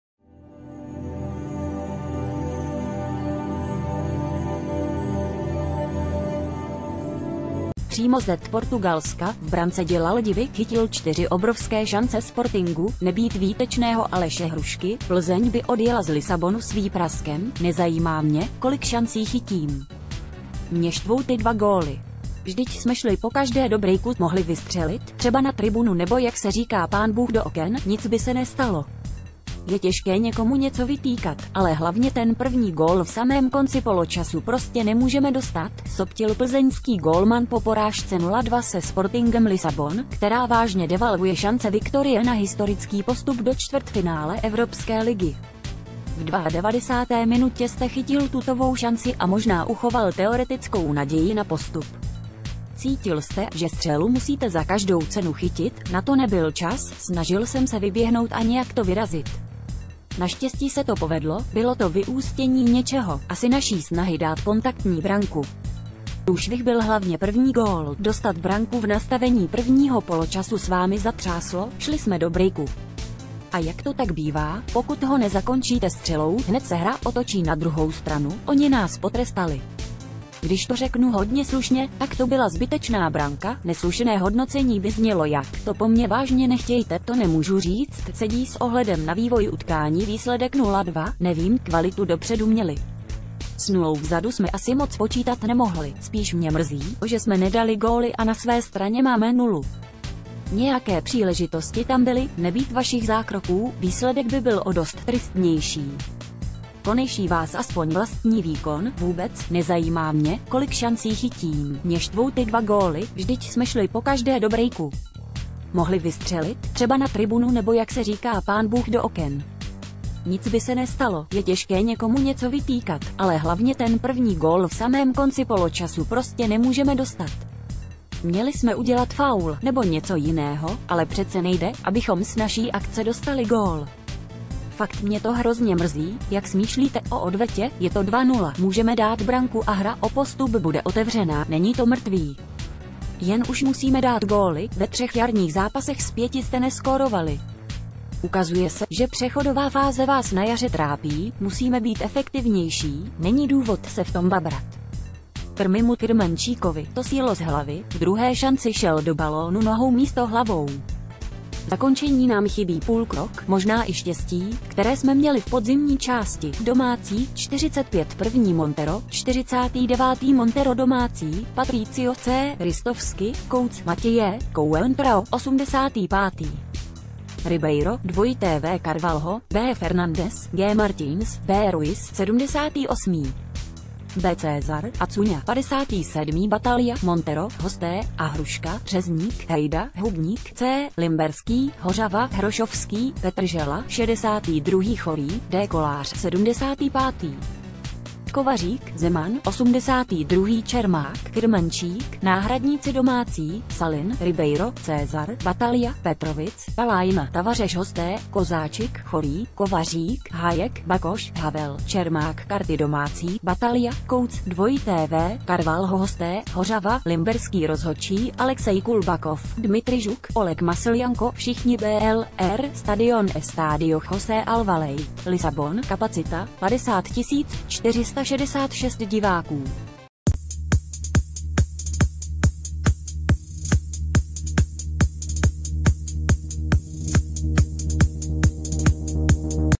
Rozbíjející se zprávy | Fantom Hruška: Postup? Není to mrtvý, ale musíme být efektivnější
PŘÍMO Z PORTUGALSKA | V brance dělal divy, chytil čtyři obrovské šance Sportingu. Nebýt výtečného Aleše Hrušky, Plzeň by odjela z Lisabonu s výpraskem. „Nezajímá mě, kolik šancí chytím. Mě štvou ty dva góly. Vždyť jsme šli pokaždé do brejku. Mohli vystřelit, třeba na tribunu nebo jak se říká pánbůh do oken. Nic by se nestalo. Je těžké někomu něco vytýkat, ale hlavně ten první gól v samém konci poločasu prostě nemůžeme dostat,“ soptil plzeňský gólman po porážce 0:2 se Sportingem Lisabon, která vá...